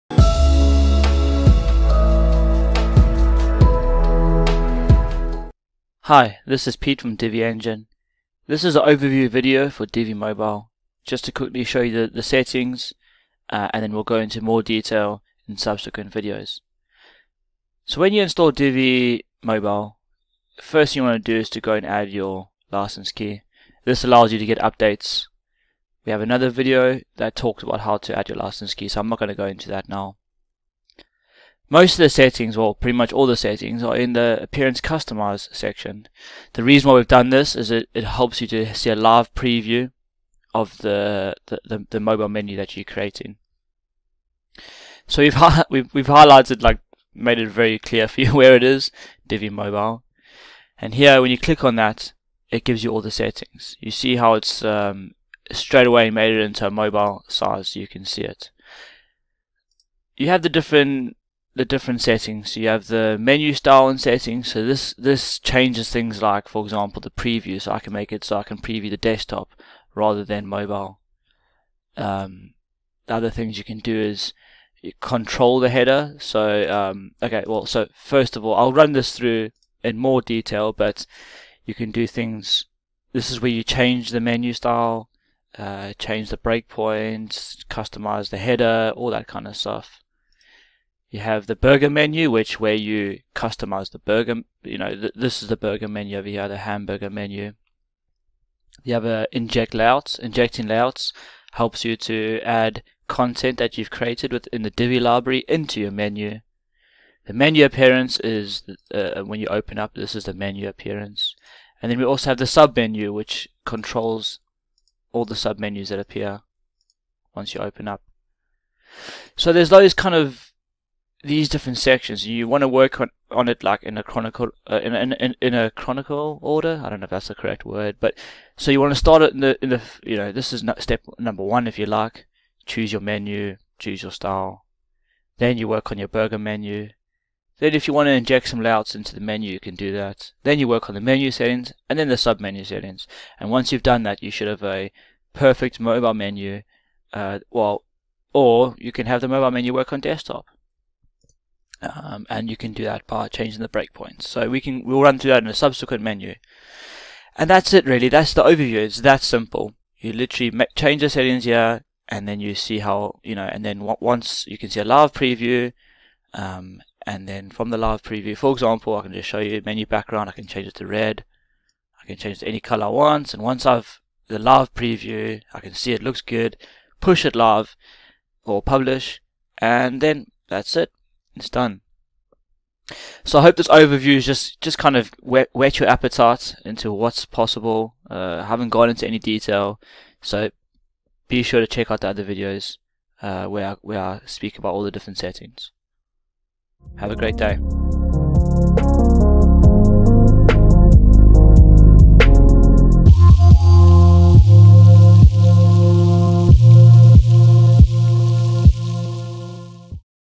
Divi Mobile - Plugin Overview
Divi Mobile. An overview of our plugin settings. Using the Divi mobile customizer settings.